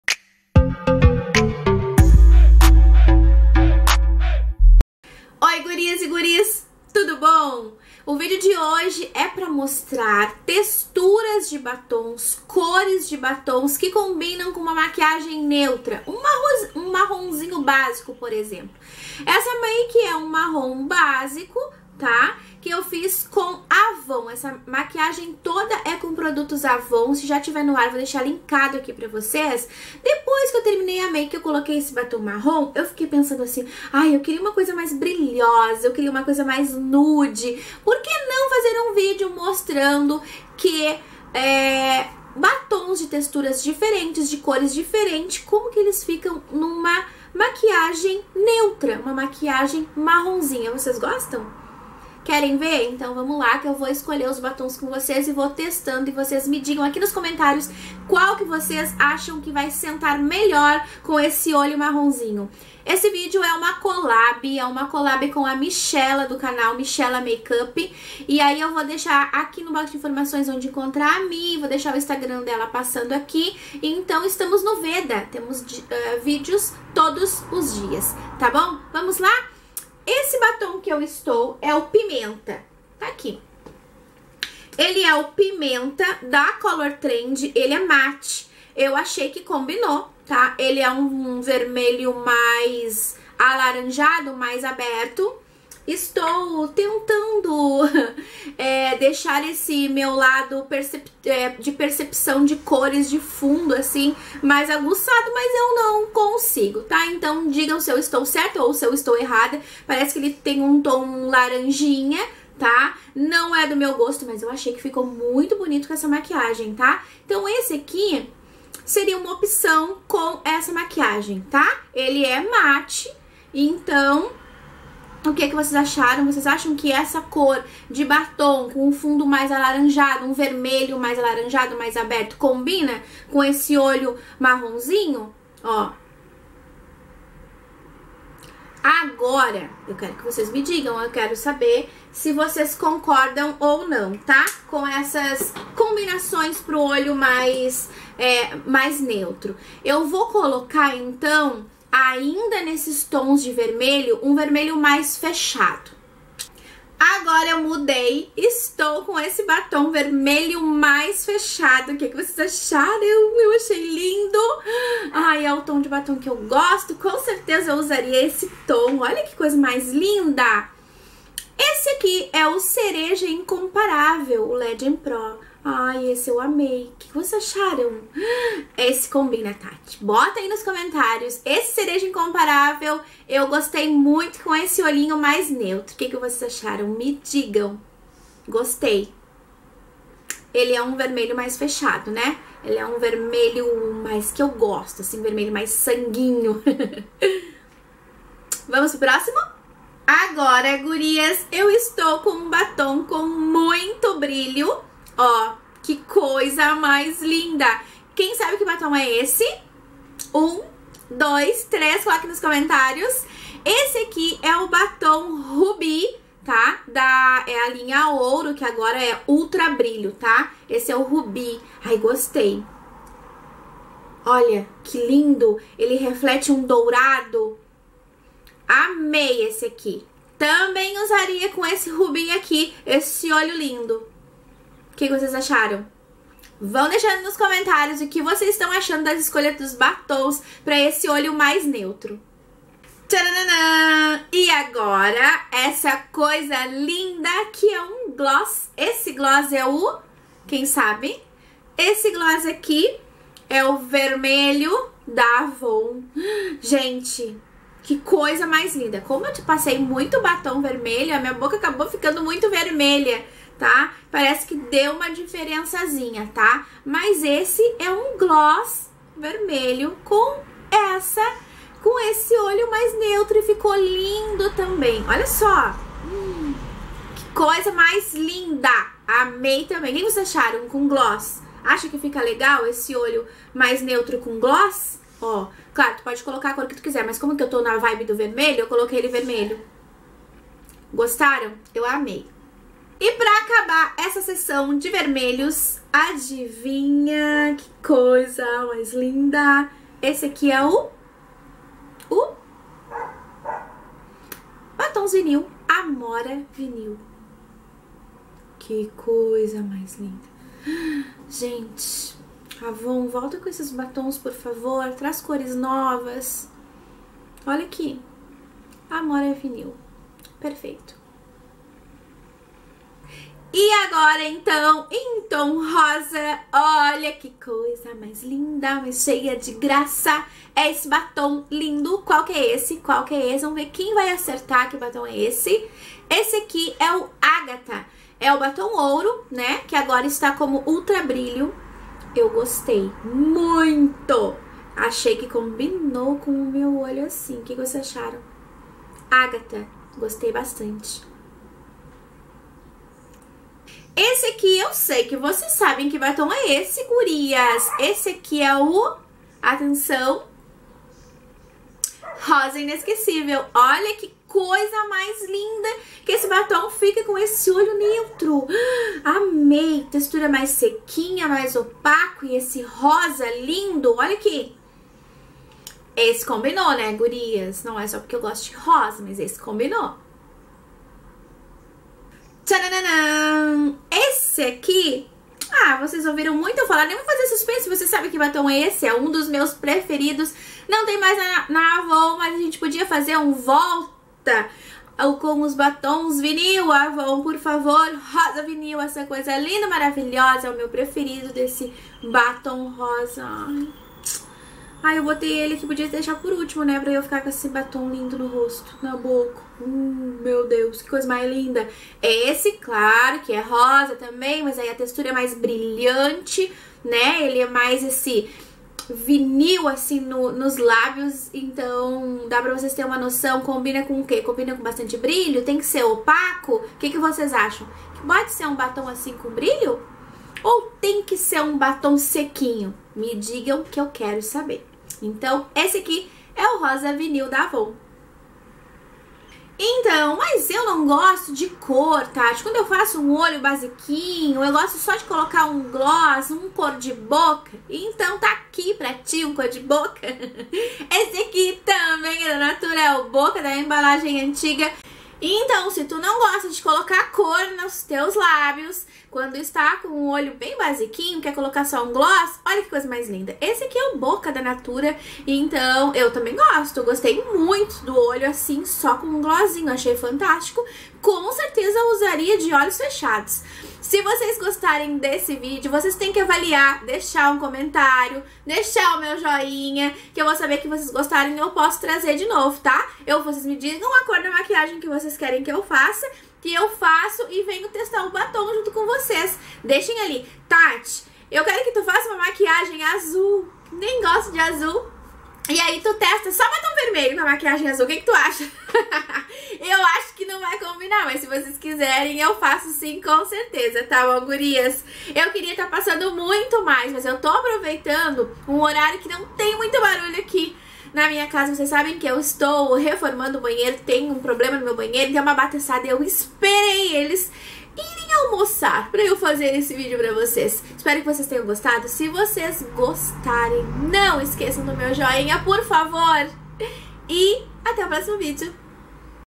UMA MAKE BASICA E VARIOS BATONS
Espaço natura Digital:
Oi Gurias e guris! Tudo bom!?
Espero que aproveitem o vídeo se tiverem
algumas duvida só deixar nos comentários
que eu respondo ou ate mesmo no instagram.

Batons que coloquei neste video
PIMENTA DA COLOR TREND MATTE AVON
CEREJA INCOMPARAVEL LEGEND PRÓ AVON
RUBY ULTRABRILHO ANTIGO OURO AVON
GLOSS VERMELHO AVON TRUE AVON
AMORA VINIL AVON (NÃO TEM MAIS EM CATALOGO)
AGATHA ULTRABRILHO ANTIGO OURO
ROSA INESQUECIVEL LEGEND PRO AVON
ROSA VINIL NÃO TEM MAIS EM CATALOGO
GLOSS BOCA DA NATURA EMBALAGEM ANTIGA